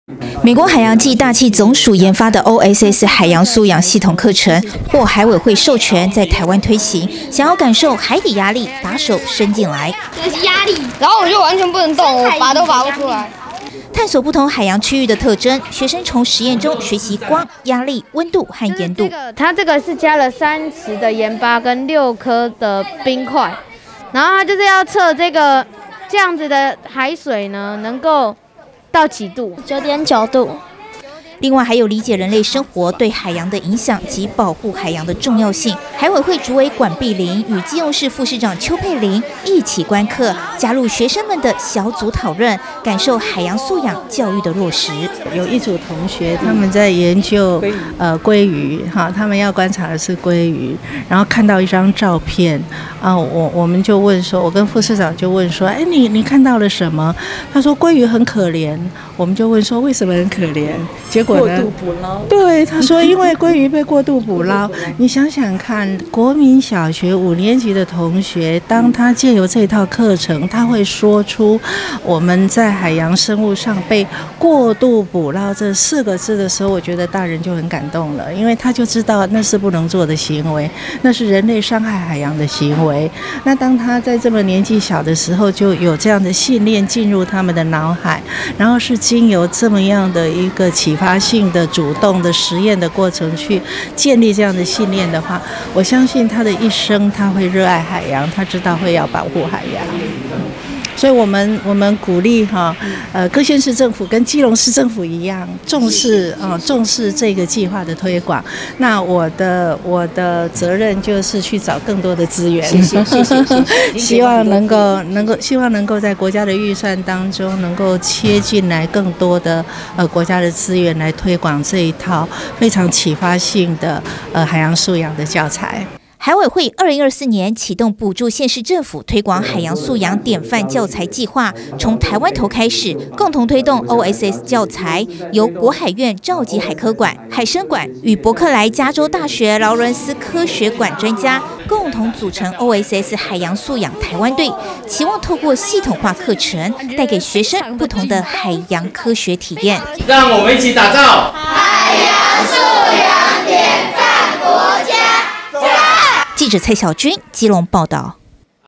海洋 教育|OSS海洋素養系統課程 打造海洋素養典範國家
基隆建德國小示範OSS海洋素養課程 海委會主委力挺扎根希望遍地開花

（中嘉新聞記者 蔡曉君/基隆採訪報導）為讓台灣成為海洋素養典範國家，海洋委員會補助縣市政府推廣海洋科學教材「OSS」，期盼孩童從小就能建立正確海洋知識及環保意識。5/30海洋委員會管碧玲主委率領國海院陳建宏院長等人，前往基隆建德國小觀課，同時與基隆市副市長邱佩琳一起見證OSS在地落實情況。

在基隆市建德國小辦理的OSS入校教學與公開課，由葉芸菁老師以雙語教學模式帶領同學學習「光、壓力、溫度和鹽度」，並探索不同海洋區域的特徵，學生們通過實驗了解光線、壓力、溫度和鹽度對海洋環境的影響；林展寬老師則以「棲息地的關聯」單元，幫助學生了解生物如何在多樣化的棲息地中形成適應性，學生們通過角色扮演和模擬活動，體驗不同生物在海洋環境中的生存策略；李明霞老師透過「人與海洋的關係」課程，使學生認識人類與海洋的各種相互關聯，通過討論和小組合作，學生們理解人類活動對海洋的影響及保護海洋的重要性。

這套OSS(Ocean Science Sequence)海洋素養系統課程，由美國海洋暨大氣總署研發，海委會於2023年取得美國柏克萊加州大學授權，在台灣翻譯與推廣OSS/G3-G5(國小)及OSS/G6-G8(國中)教材，並透過補助縣市政府推動OSS。

海委會主委管碧玲與基隆市邱佩琳副市長均親臨現場參與工作坊並與社群教師座談。管主委表示：基隆老師將OSS課程融入雙語教學也令人驚艷，也看到學生在課程中全心投入在探索海洋科學的過程，這也落實海委會期望透過OSS這套教材與國際接軌，進而達成打造海洋素養典範國家的目標。同時繼續推動這項中央與地方政府的合作計畫，共同推動海洋素養教育，讓我們的孩子能夠透過輕鬆有趣的課程，持續深化海洋知識和海洋意識。

海委會於2024年啟動補助縣市政府推廣海洋素養典範教材計畫，從台灣頭開始，共同推動OSS教材，由國海院召集海科館、海生館與柏克萊加州大學勞倫斯科學館專家，共同組成OSS海洋素養台灣隊，期望透過系統化課程帶給學生不同的海洋科學體驗，共同打造海洋素養典範國家。

海洋科學序列(Ocean Science Sequence)